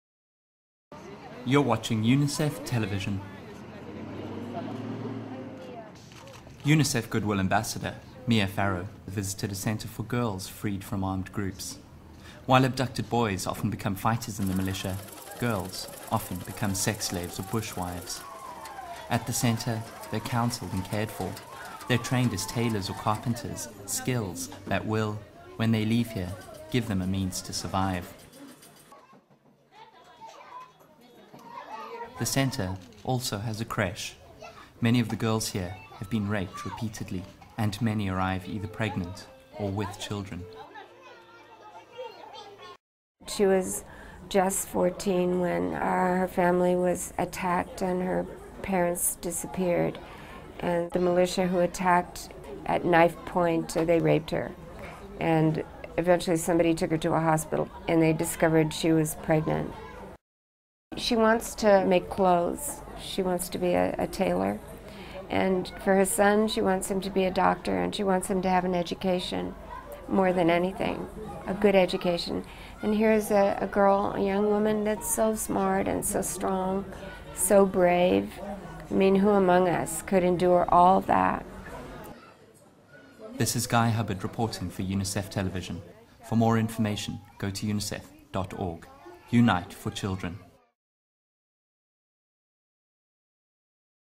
Mia Farrow visits girls affected by gender-based violence in DR Congo
UNICEF - 26 March 2012 - UNICEF correspondent Guy Hubbard reports on UNICEF Goodwill Ambassador Mia Farrow's visit to a transit centre for abused girls in Bukavu, Democratic Republic of the Congo.